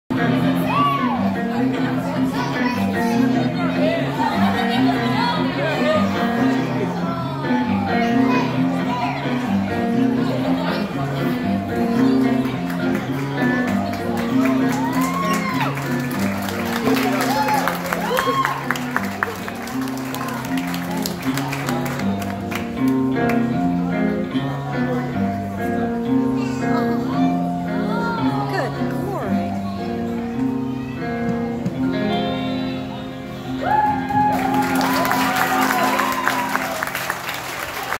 Hair Twirling at the City Museum
Circus act. Hanging hair twirling woman